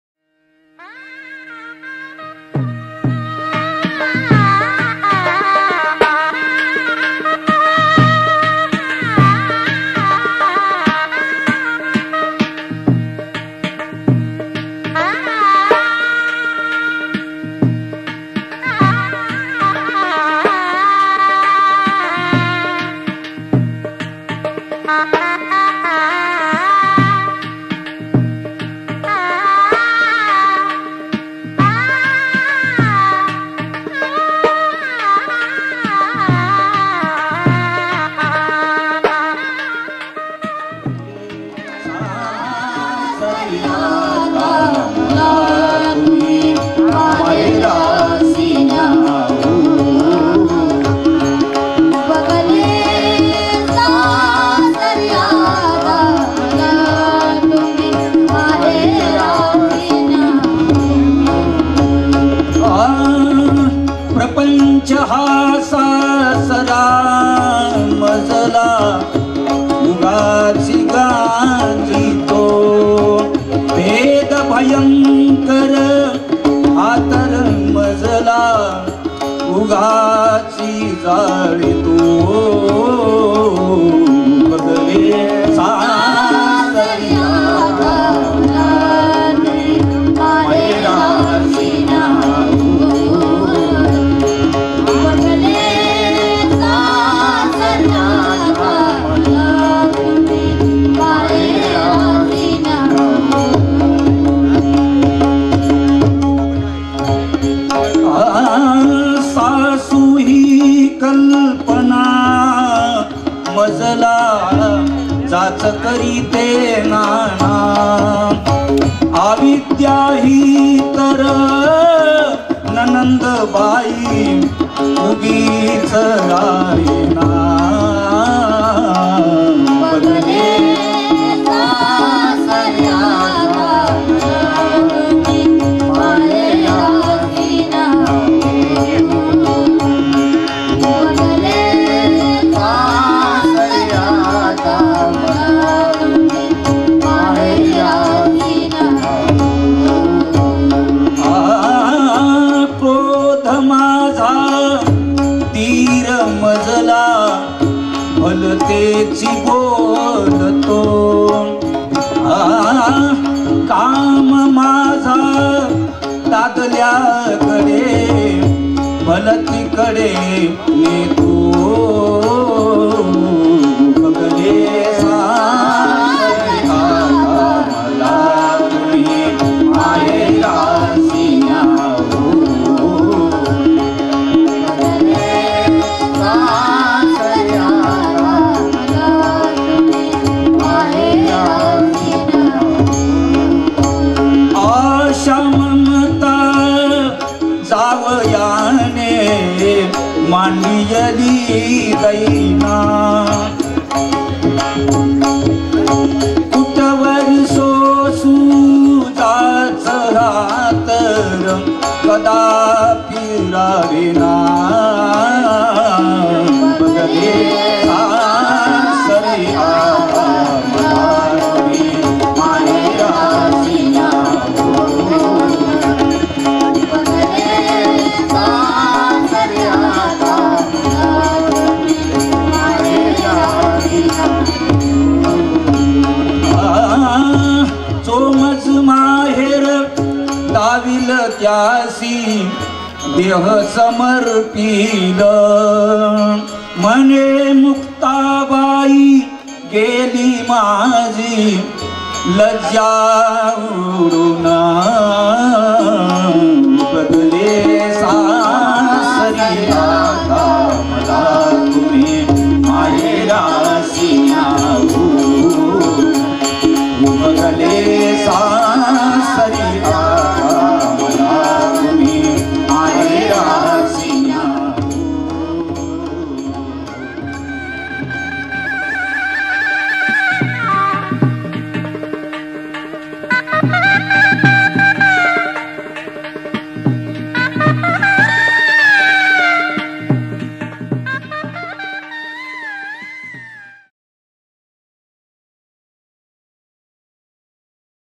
Abhangvani Gangakhed Sugar part 13
Bhajan Spardha ,Participated Contestants video clips.
Dated;9th-10th-11th-August/2015